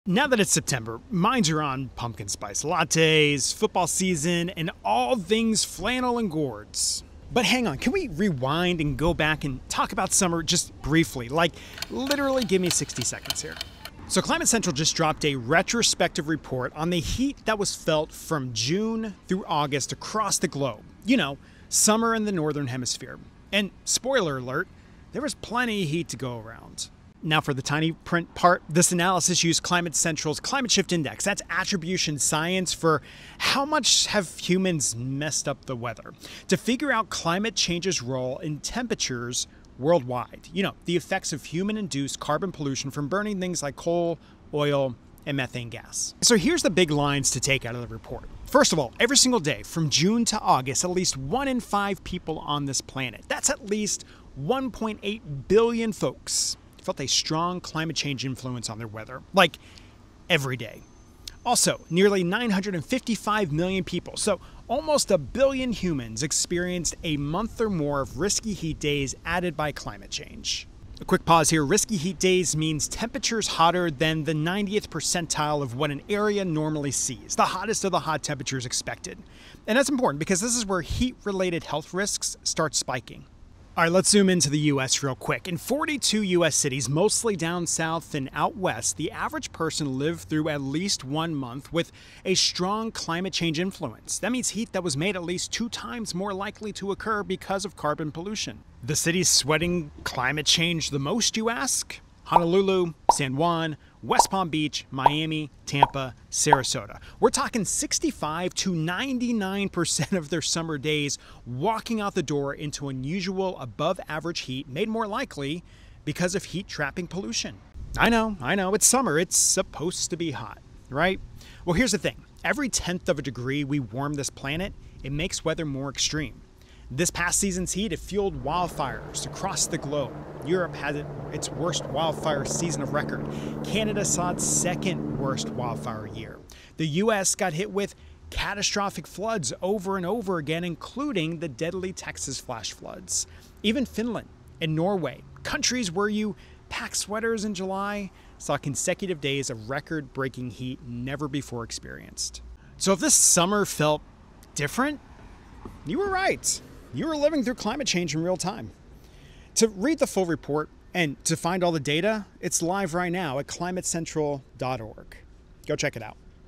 If summer 2025 felt different, here's how climate change played a role
If this summer's heat felt unusual, you weren't imagining it.
Every single day, between June and August, 1 in 5 people worldwide felt climate change's influence on their weather.